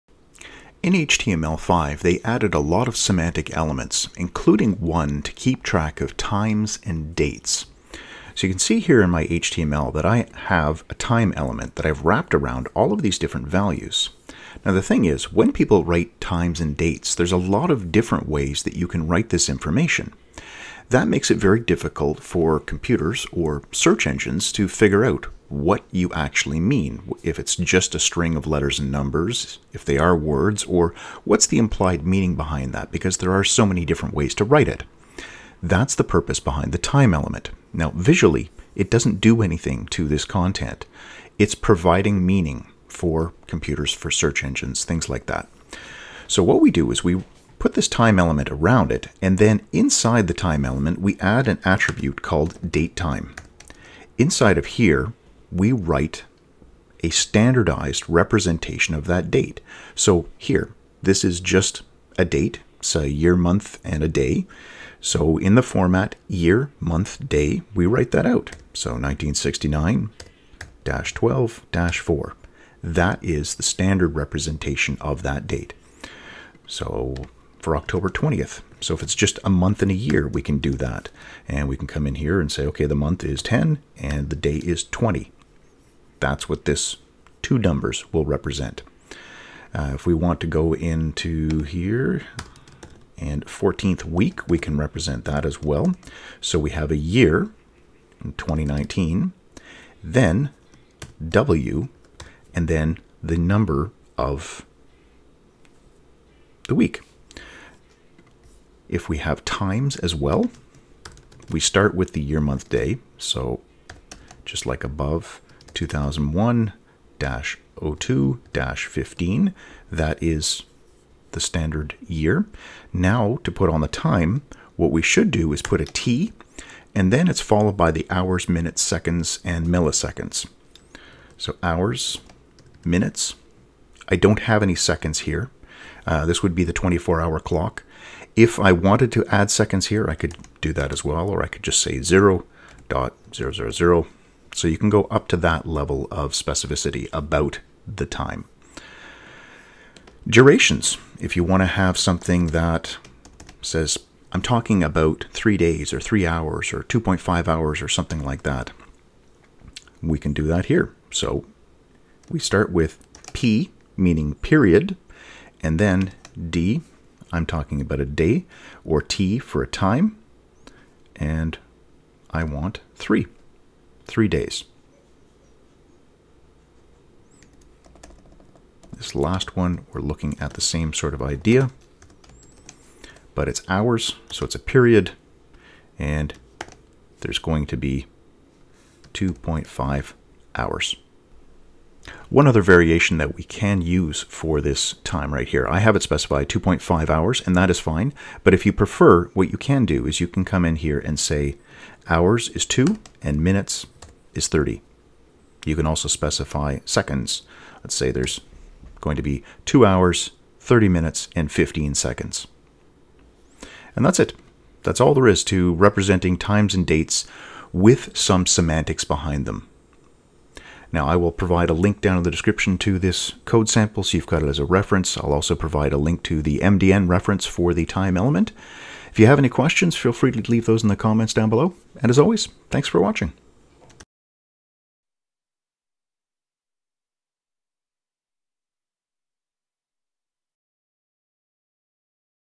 HTML5 time Element
This tutorial talks about how to use the HTML time element and the datetime attribute to add semantic value to dates and times that are in your HTML content.

One thing that was left out of the tutorial was timezones. If you have a date-time value and want to include the time zone you can add a plus or minus sign followed by a HH:MM value.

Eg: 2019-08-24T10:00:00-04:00
represents 24 August 2019 @ 10am (4 hours behind Greenwich mean time)